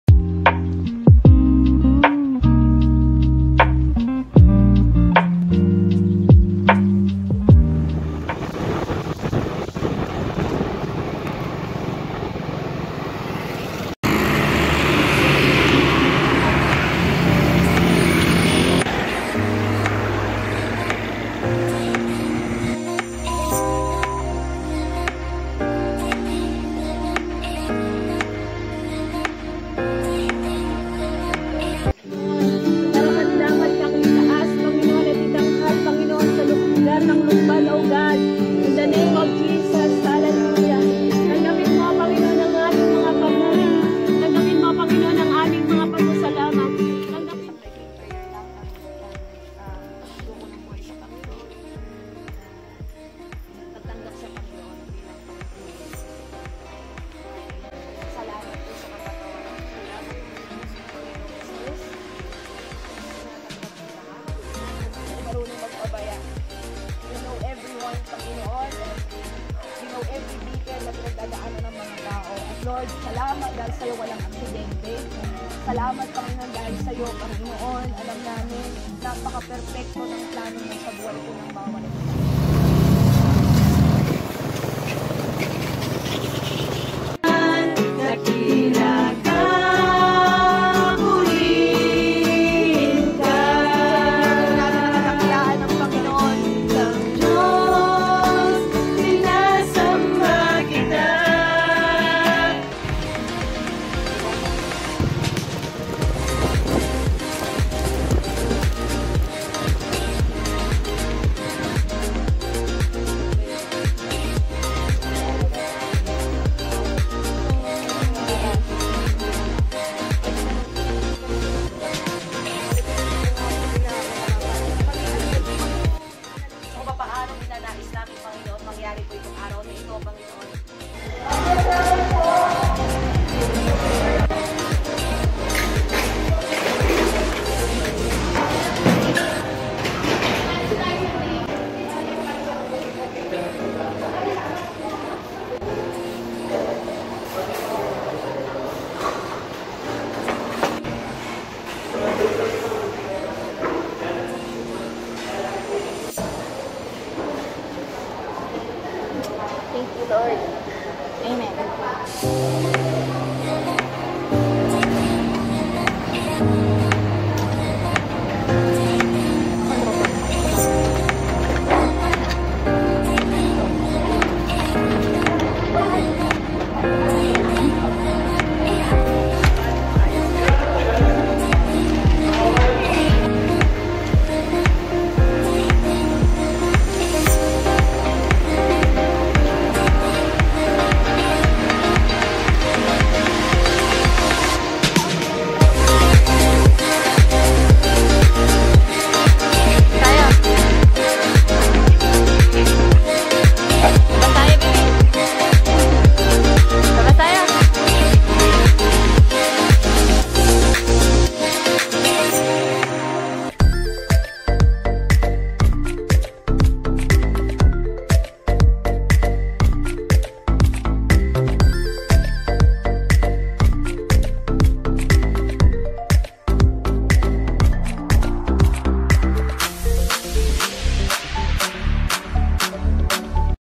LUCBAN IS FOR JESUS | VLOG#23
PRAY MORE, LOVE MORE, JESUS MORE IN YOUR LIFE!
We went to Lucban to share the good news, the gospel. We all need Jesus!

If you have any prayer request just DM of my IG, willing to pray for you!